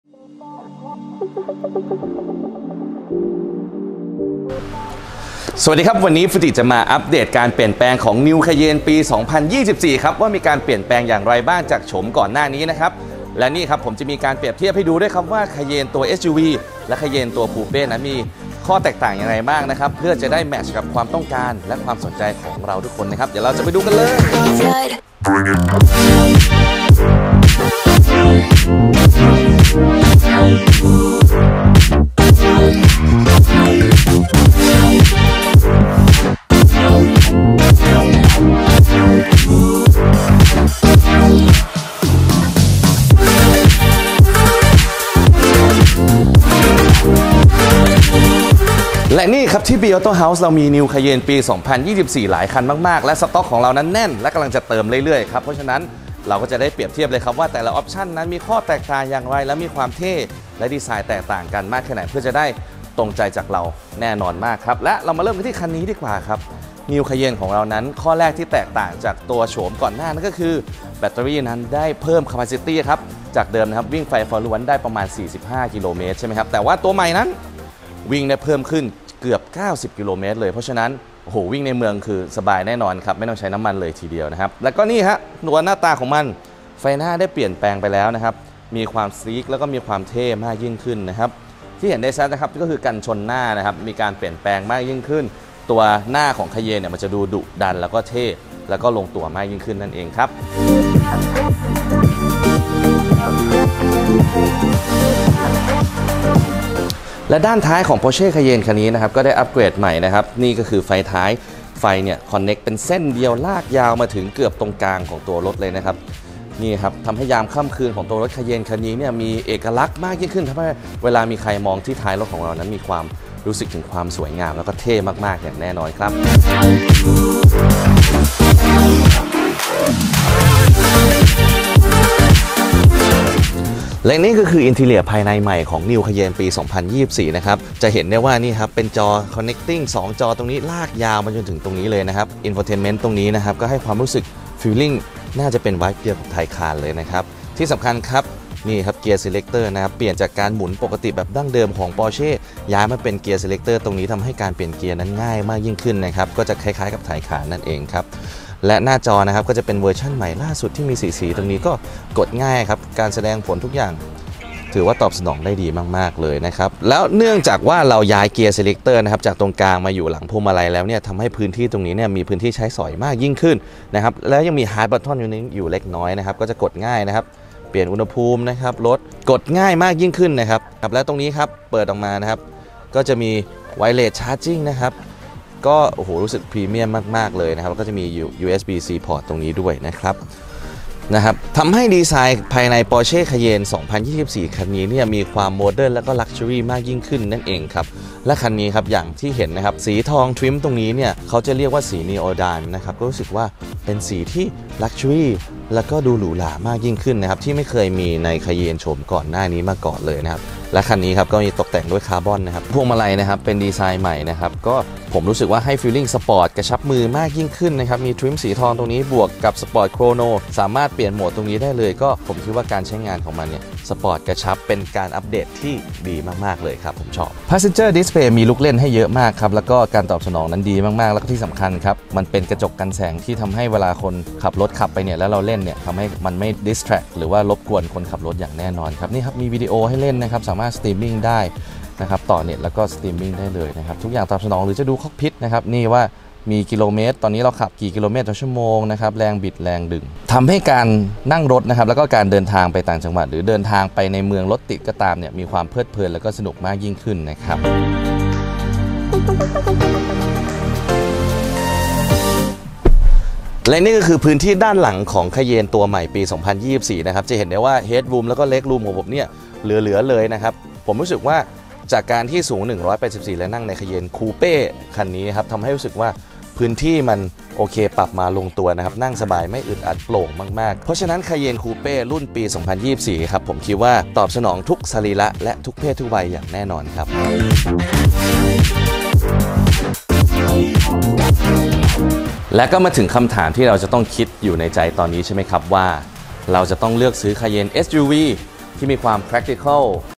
EP-43 รีวิวให้เห็นกันชัดๆ! New Porsche Cayenne Facelift 2024
รีวิวให้เห็นกันชัดๆ! New Porsche Cayenne Facelift 2024 Cayenne Coupe vs SUV
กับเซลล์ฟูจิ Sales Executive ของ B Autohaus ทั้งแบบ SUV และ Coupe พาชมทุกมุมของตัวรถที่มีการปรับแต่งใหม่จะมีอะไรกันบ้าง คลิกเพื่อชมวีดีโอได้เลยครับ